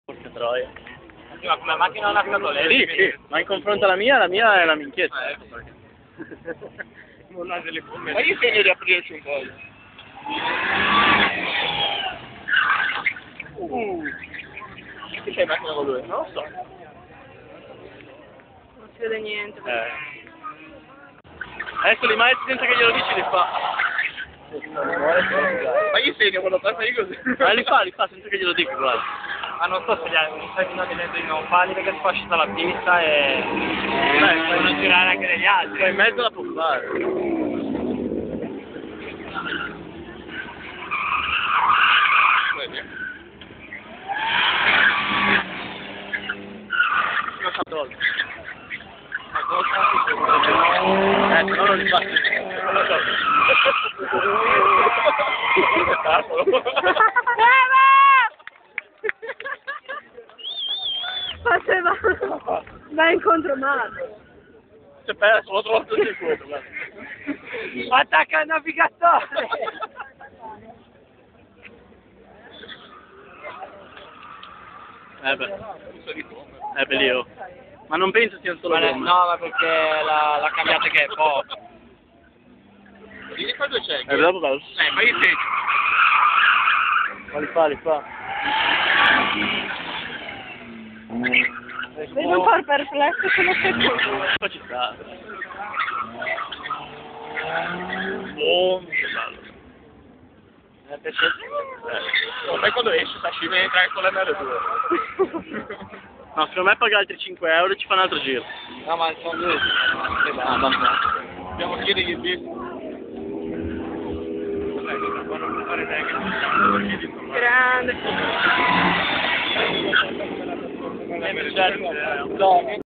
Mercedes SLK 200 K ginkana in Colmata a Chiavari (GE) anno 2005 organizzato dal ZTC
vecchio video storico di una ginkana , organizzata dall'allora ZTC (Zena Tuning Club) nel piazzale della Colmata a Chiavari  ((GE)) ricorreva l'anno 2005 io con un SLK 200 K Stock faccio lo scemo nel percorso , anche in questo caso il video risulta sgranato per via del telefono all'epoca molto obsoleto